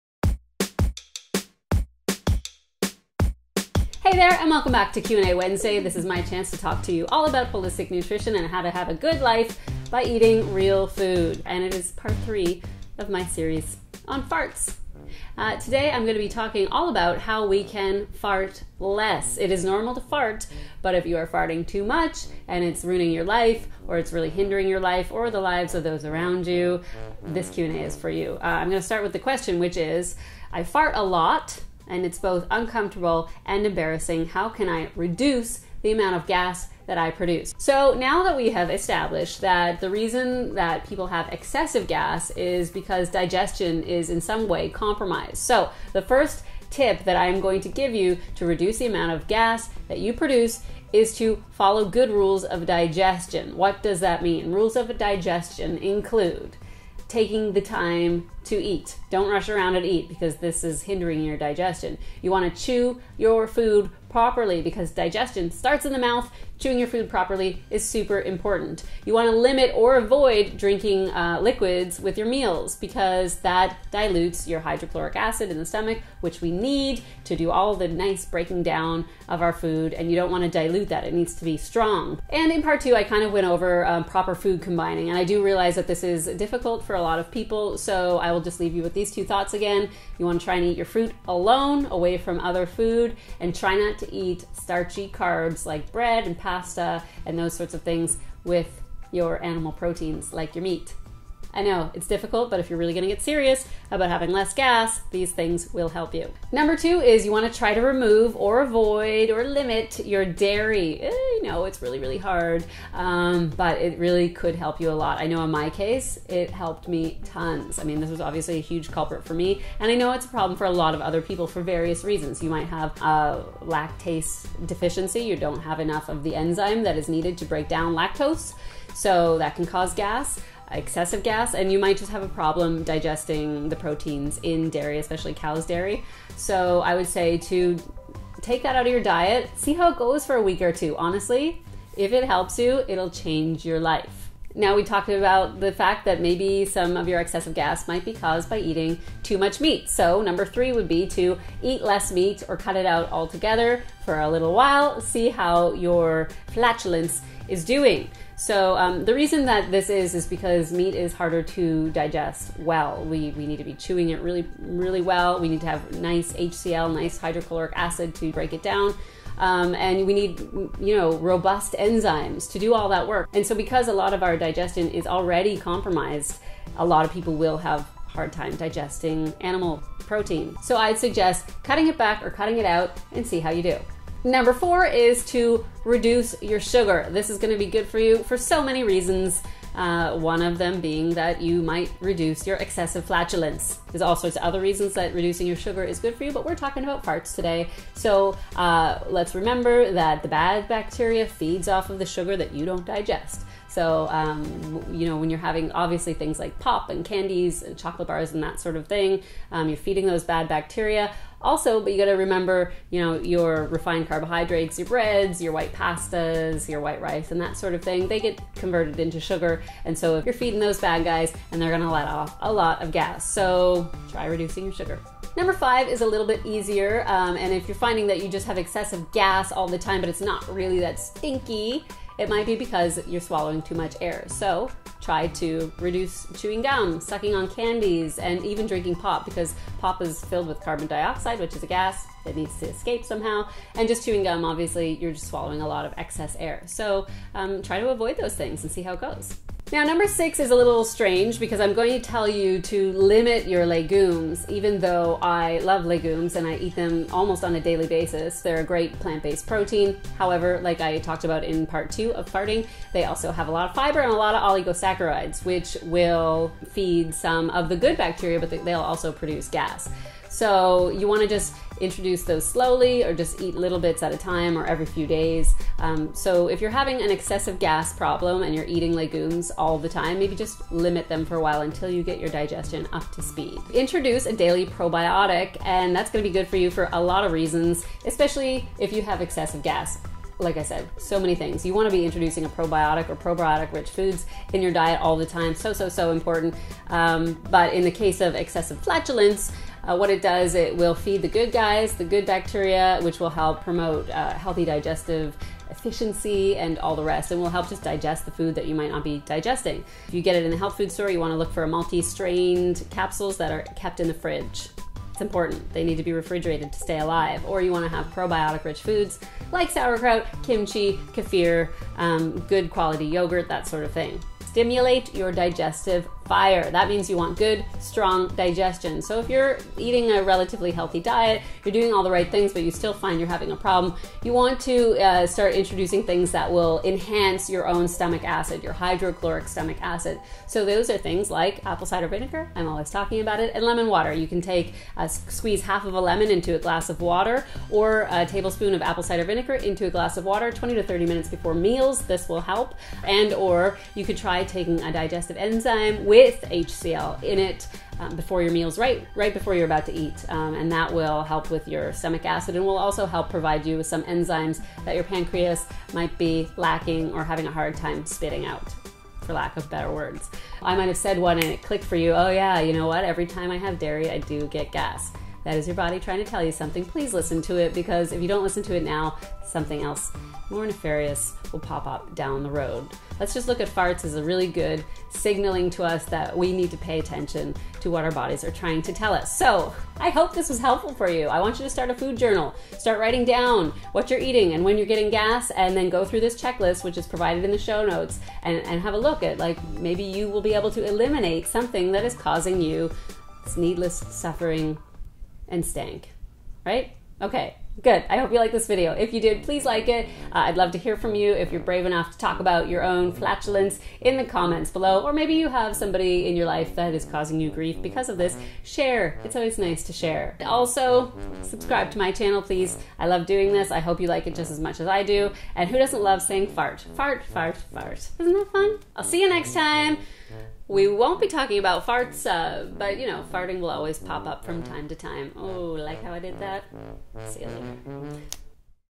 8 Natural Ways To Reduce Excessive Gas with Holistic Nutritionist, Kim D'Eon
In this video, holistic nutritionist, Kim D'Eon outlines 8 easy ways to reduce excessive gas.

Here's a quick checklist of things to consider to help you be LESS gassy, Lassy!

1. Follow good RULES OF DIGESTION: (take time to eat, chew your food properly, limit/avoid liquids with meals, practice good food combining)
2. Reduce/avoid DAIRY (especially Cow's dairy)
3. Reduce/avoid MEAT which is tough to digest (especially red meat and pork)
4. Reduce/Avoid SUGAR
5. Reduce chewing GUM and sucking on CANDIES.
6. Limit LEGUMES (peas, beans, lentils, chickpeas, etc) until your digestion catches up to this higher amounts of this wonderful plant-based protein
7. Include PROBIOTICS daily (either from foods like sauerkraut, kimchi, miso, tempeh or kefir)
8. Stimulate your DIGESTIVE FIRE with apple cider vinegar or lemon water or introduce DIGESTIVE ENZYMES (with HCl)